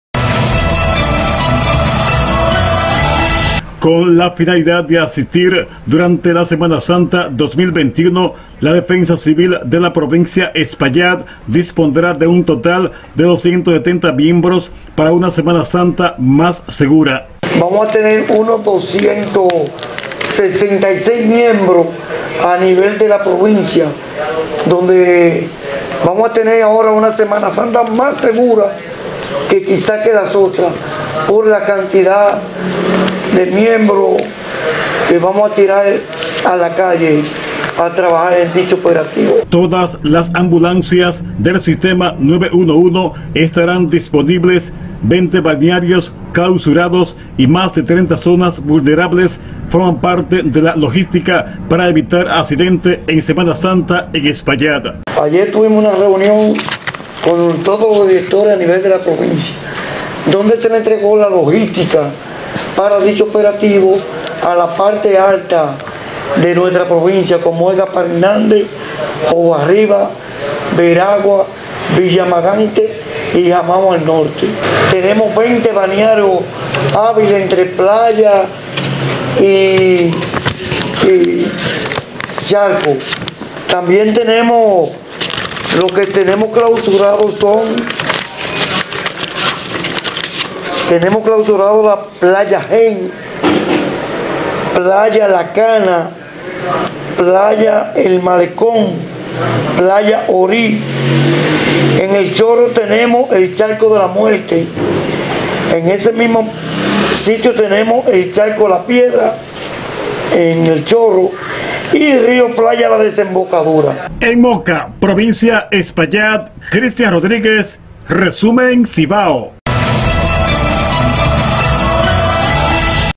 La defensa civil en la provincia Espaillat ya esta lista para asistir en semana santa 2021
Defensa Civil